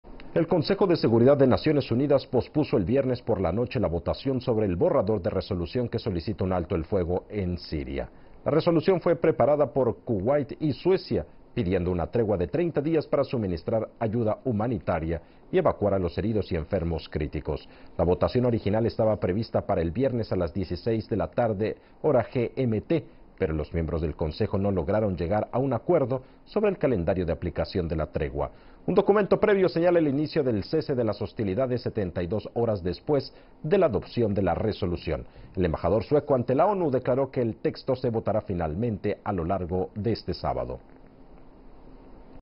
El Consejo de Seguridad de la ONU retrasa la votación sobre el alto el fuego en Siria
El Consejo de Seguridad de Naciones Unidas pospuso el viernes por la noche la votación sobre el borrador de resolución que solicita un alto el fuego el Siria.